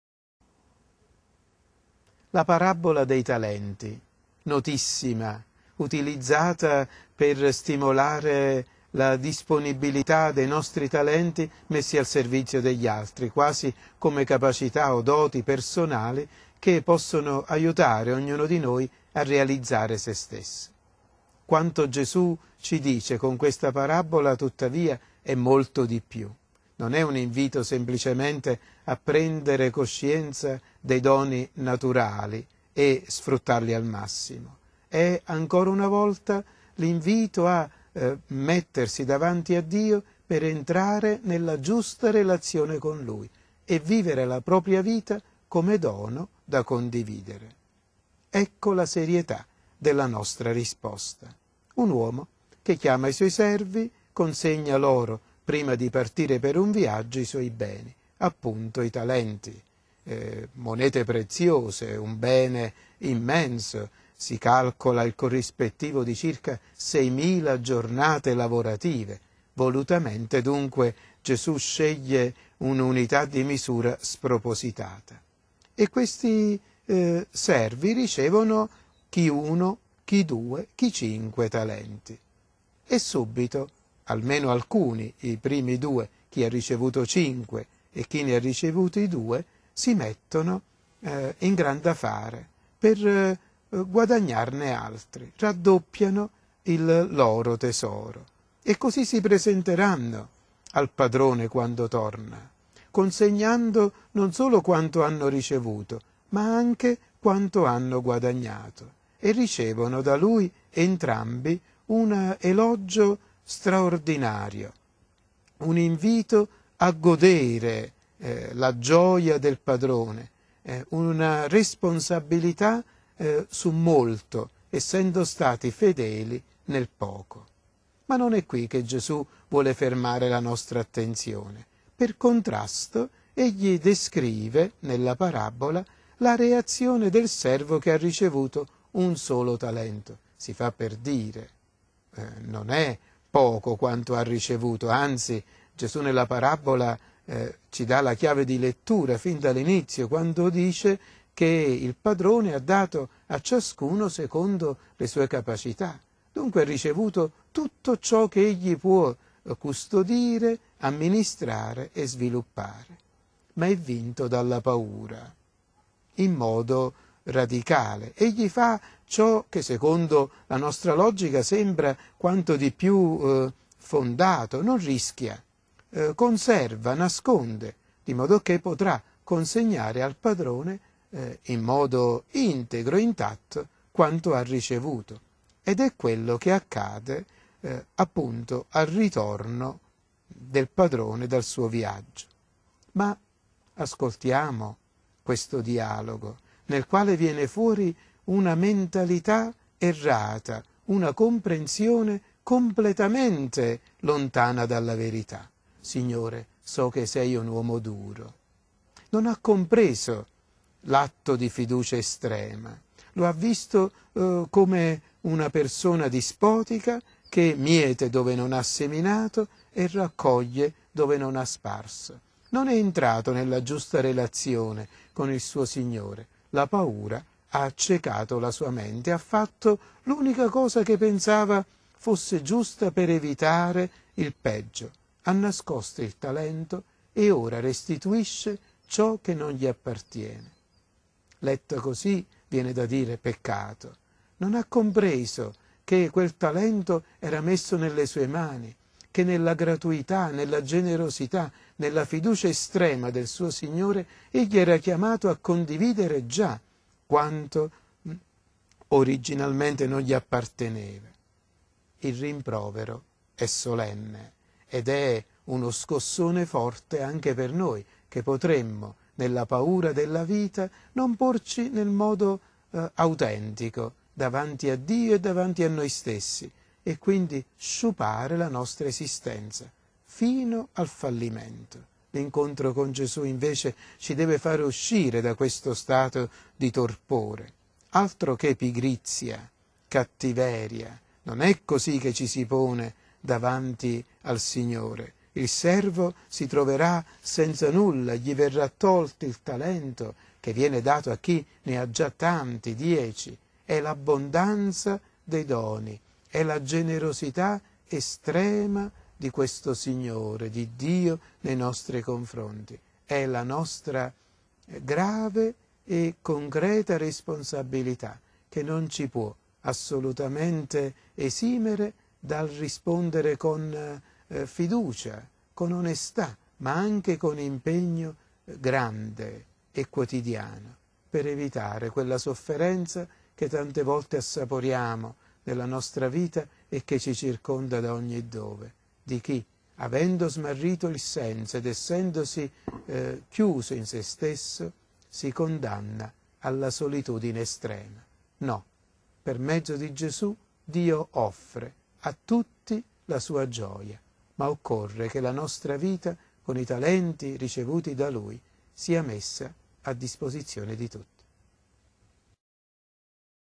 13novembre2011.wmv
Il Vangelo Commentato dal vescovo nel web_XXXIII Domenica del Tempo Ordinario 2011_Mt 25, 14-30